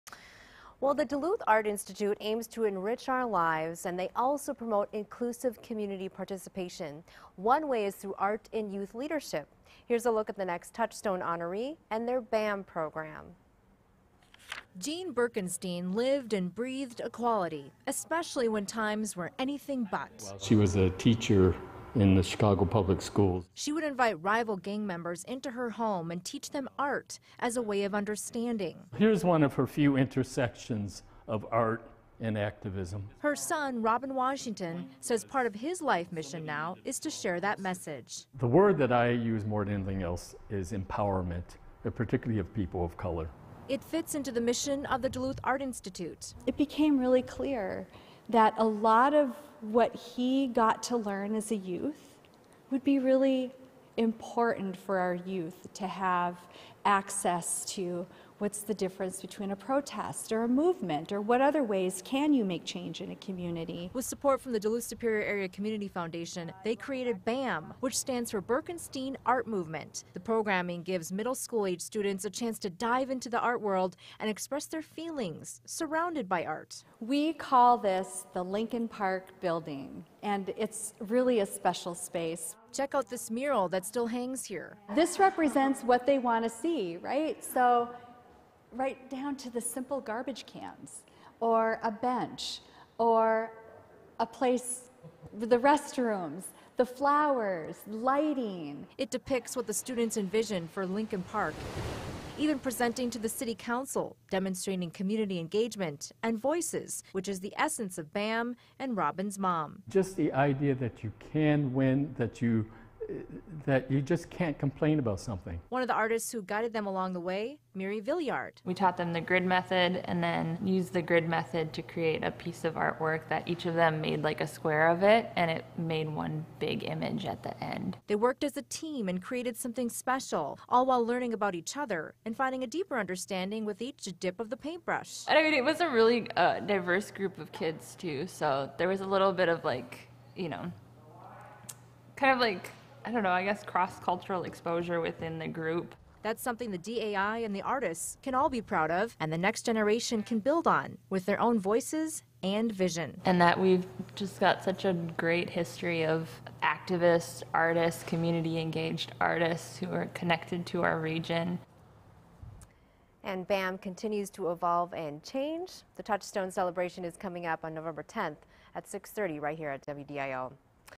Touchstone 2022: Duluth Art Institute's Birkenstein Arts Movement
Equality through arts. See how the Duluth Art Institute is empowering students to use their voice and make change. The Birkenstein Arts Movement (BAM) brings lessons and skills to the next generation of engaged citizens.

The program is supported in part by the Duluth Superior Area Community Foundation and is recognized in the 2022 Touchstone Celebration.

Touchstone Celebration stories produced by Renee Passal and WDIO-TV.